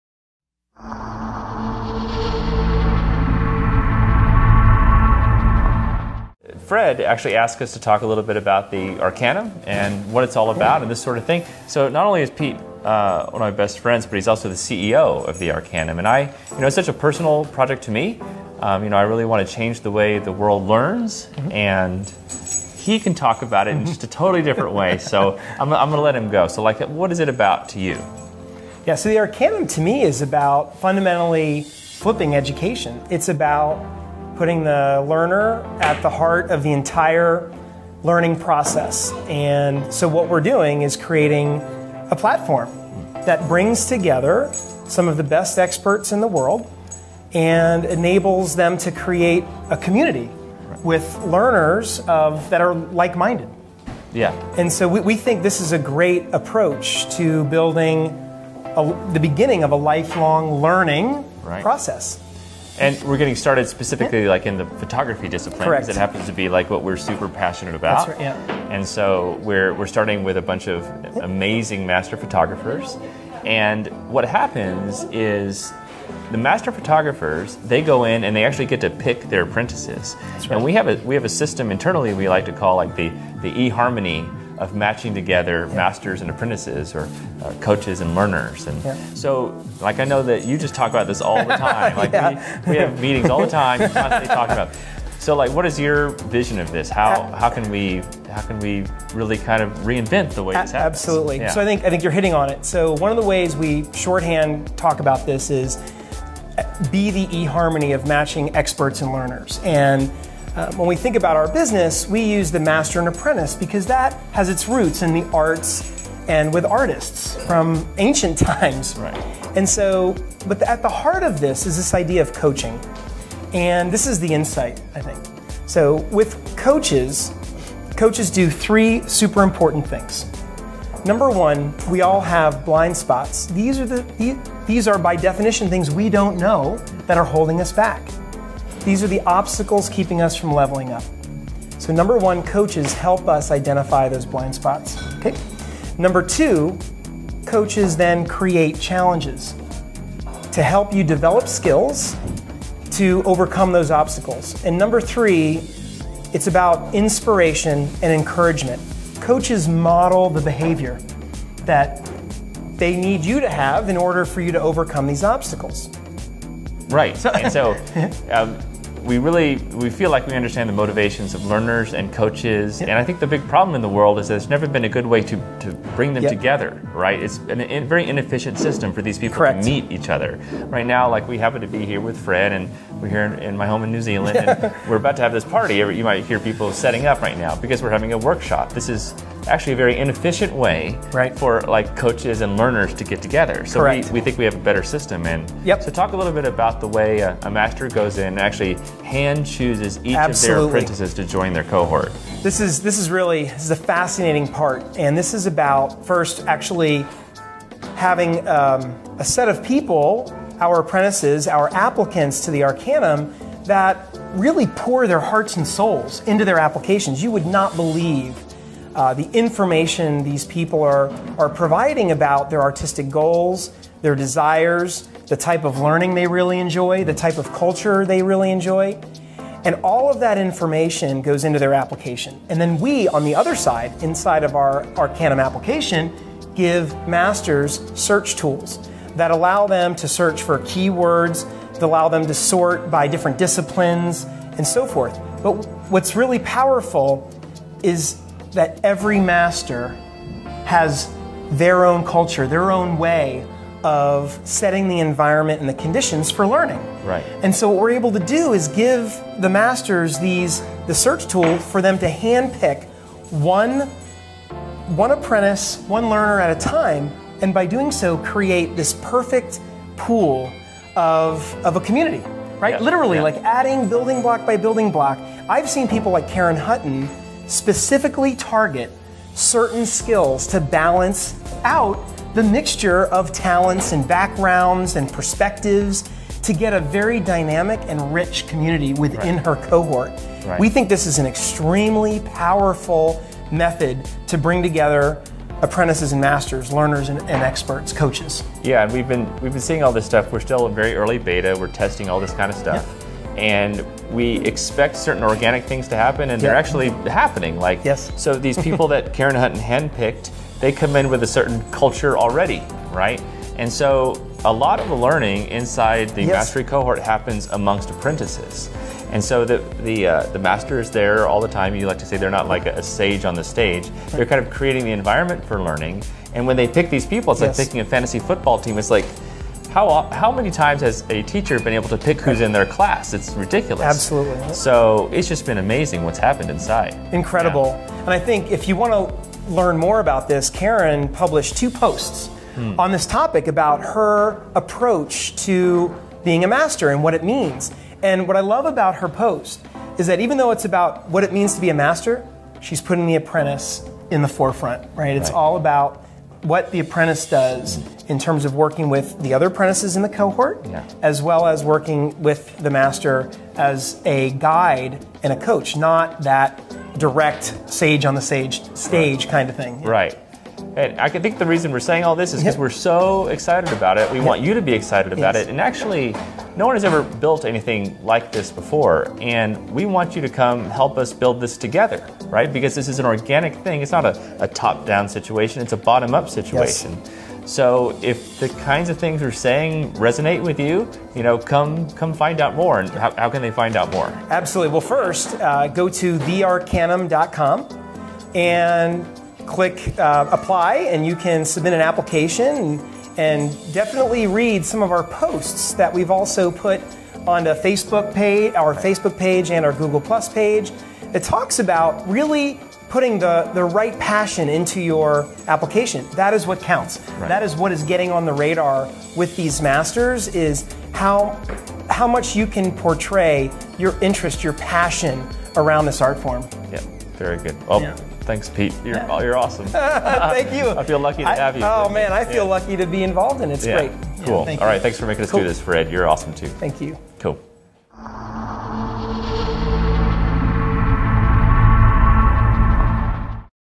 What is The Arcanum about to Trey and Pete?
Pete and Trey sit down at home to talk more about The Arcanum and many of our thoughts behind it.  Thanks Frederick Van Johnson for helping to record this and hanging out with me at our home.  Pete talks about the three important things coaches do.  Also, thanks Fred for adding that soft-porn music in the background...it's a soft touch, Fred.